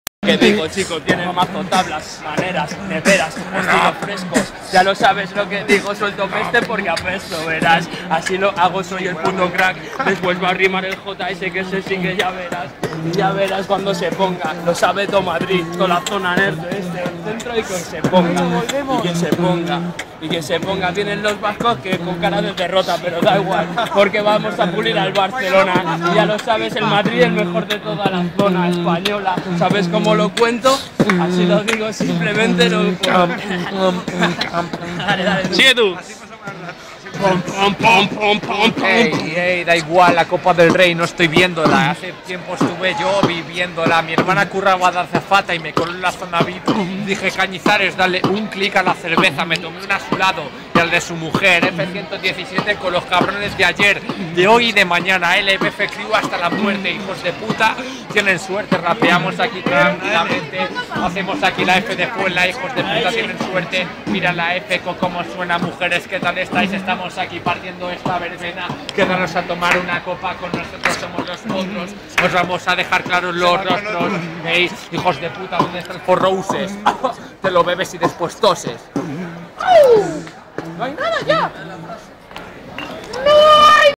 Freestyle in the street @ Huertas (Madrid) 12-02-2011 (1)
Freestyle espontaneo de JS, Javi y Oxi en Huertas